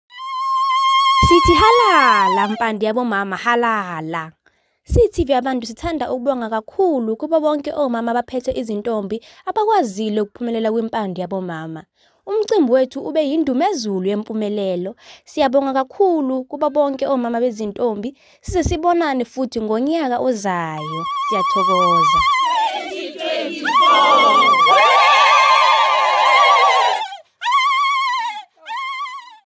impande yabo mama
impande yabo mama @TVYABANTU  is an annual spiritual ritual at Yabantu Village in south africa, where Mother  Goddesses commited in protection and care of our culture, customs and traditions